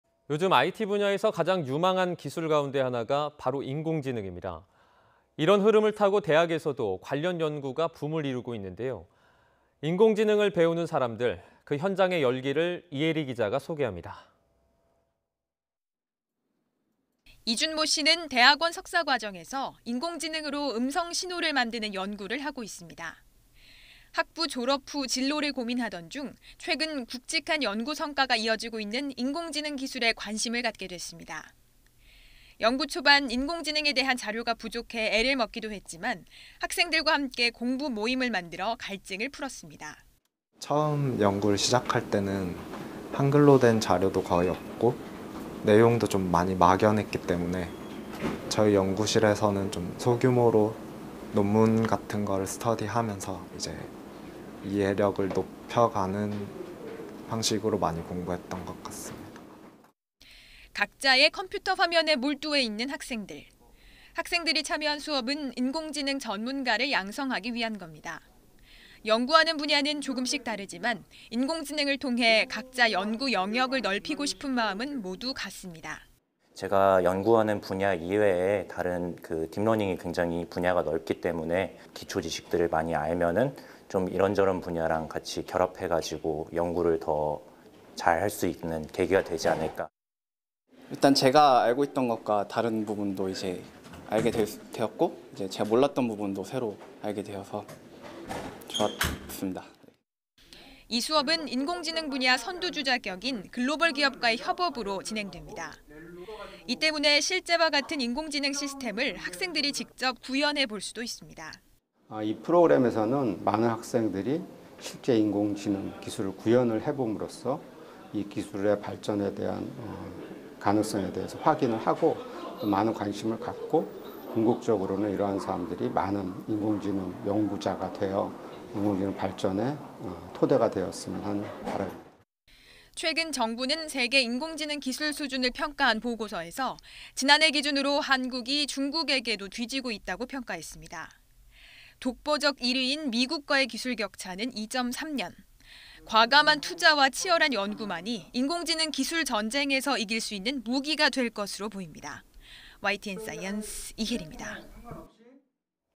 "인공지능을 배우자"…학계에 부는 바람 / YTN 사이언스
[앵커]
요즘 IT 분야에서 가장 유망한 기술 가운데 하나가 바로 인공지능입니다.

이런 흐름을 타고 대학 내에서도 관련 연구가 붐을 이루고 있는데요.

인공지능을 배우는 사람들, 그 현장의 열기를 이혜리 기자가 소개합니다.

[기자]
이준모 씨는 대학원 석사 과정에서 인공지능으로 음성 신호를 만드는 연구를 하고 있습니다.

학부 졸업 후 진로를 고민하던 중 최근 굵직한 연구 성과가 이어지고 있는 인공지능 기술에 관심을 갖게 됐습니다.

연구 초반 인공지능에 대한 자료가 부족해 애를 먹기도 했지만 학생들과 함께 공부 모임을 만들어 갈증을 풀었습니다.

[이준모 / 한양대 음성 신호 처리 석사과정 : 처음 연구를 시작할 때는 한글로 된 자료도 거의 없고 내용도 많이 막연했기 때문에 저희 연구실에서 소규모로 논문 같은 걸 공부하면서 이해력을 높여가는 방식으로 공부했던 것 같습니다.]

각자의 컴퓨터 화면에 몰두해 있는 학생들.

학생들이 참여한 수업은 인공지능 전문가를 양성하기 위한 겁니다.

연구하는 분야는 조금씩 다르지만 인공지능을 통해 각자 연구 영역을 넓히고 싶은 마음은 모두 같습니다.

[이형민 / 연세대 전기전자공학과 대학원생 : 제가 연구하는 분야 이외에 딥러닝 분야가 넓어서 기초 지식을 많이 알면 이런저런 분야와 같이 결합해서 연구를 더 잘할 수 있는 계기가 되지 않을까….]

[이용주/ 연세대 전기전자공학과 대학원생 : 제가 알고 있던 것과 다른 부분도 알게 됐고 잘 몰랐던 부분도 새로 알게 돼서 좋았습니다.]

이 수업은 인공지능 분야 선두주자 격인 글로벌 기업과의 협업으로 진행됩니다.

이 때문에 실제와 같은 인공지능 시스템을 학생들이 직접 구현해볼 수도 있습니다.

[이상윤 / 연세대 AI 연구단장 : 이 프로그램에서는 많은 학생이 실제 인공지능 기술을 구현해 봄으로써 이 기술의 발전 가능성을 확인하고 많은 관심을 갖고 궁극적으로 이런 사람들이 인공지능 연구자가 되어 인공지능 발전에 토대가 되었으면 하는 바람입니다.]

최근 정부는 세계 인공지능 기술 수준을 평가한 보고서에서 지난해 기준으로 한국이 중국에게도 뒤지고 있다고 평가했습니다.

독보적 1위인 미국과의 기술 격차는 2.3년.

과감한 투자와 치열한 연구만이 인공지능 기술 전쟁에서 이길 수 있는 무기가 될 것으로 보입니다.